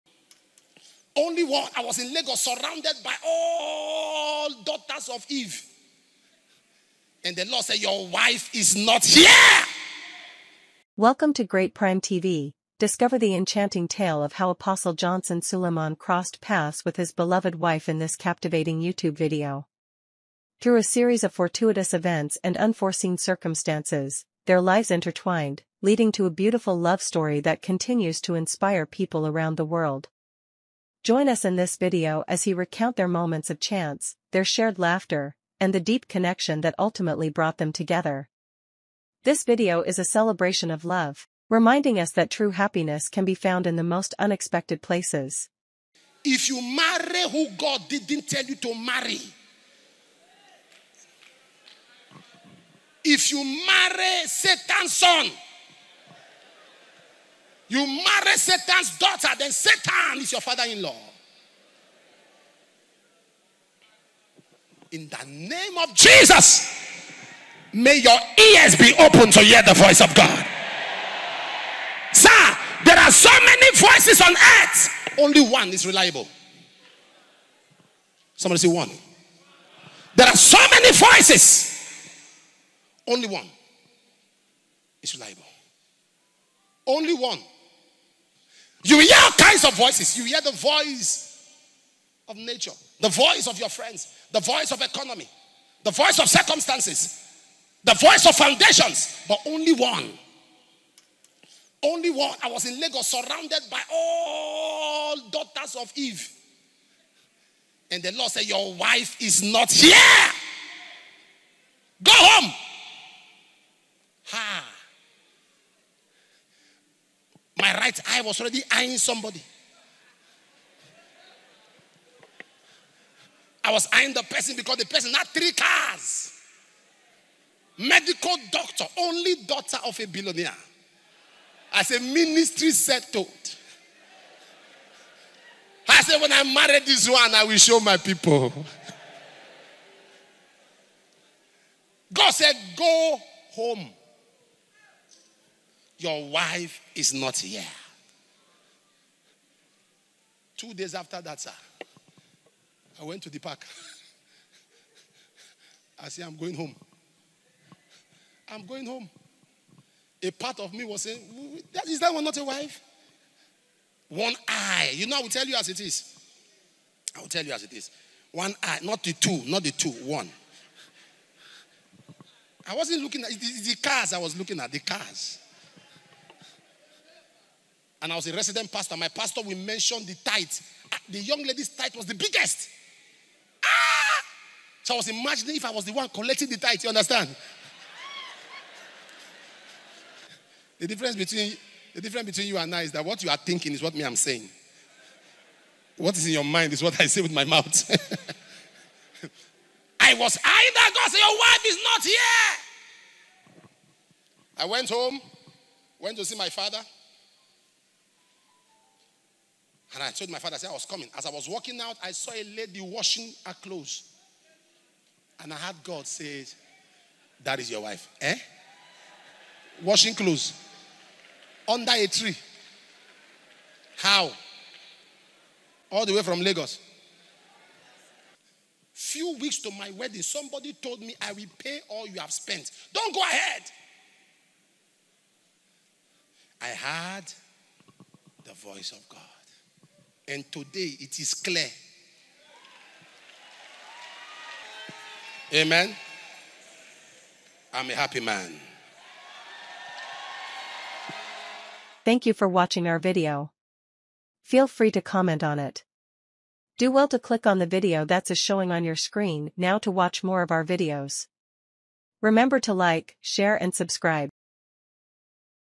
How I Met My Wife Dr Lizzy Johnson Suleman by Apostle Johnson Suleman
Discover the enchanting tale of how I crossed paths with my beloved wife in this captivating YouTube video. Through a series of fortuitous events and unforeseen circumstances, our lives intertwined, leading to a beautiful love story that continues to inspire. Join me as I recount the moments of chance, the shared laughter, and the deep connection that ultimately brought us together. This video is a celebration of love, reminding us that true happiness can be found in the most unexpected places.